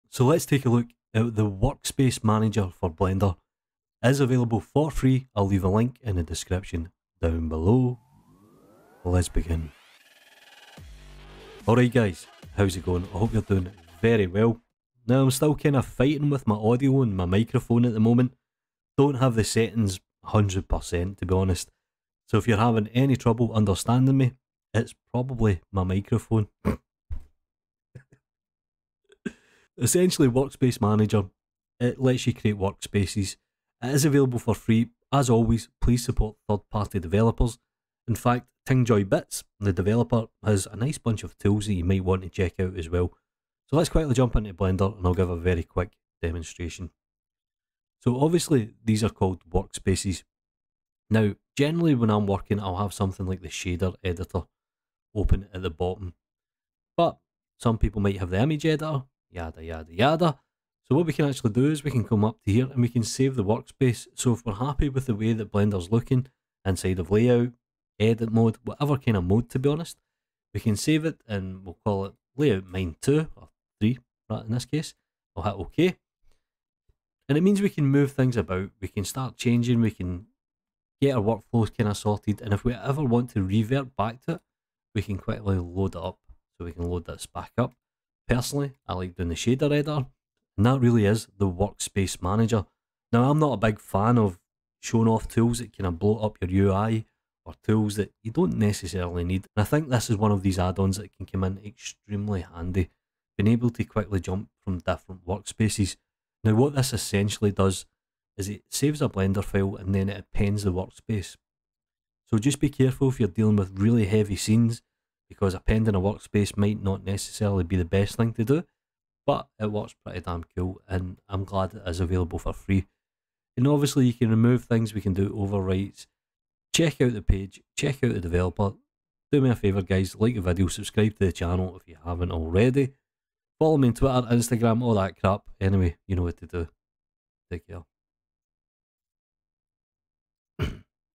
Workspace Manager for Blender Add-on
A great little add-on for saving and restoring workspaces.
LINKS BELLOW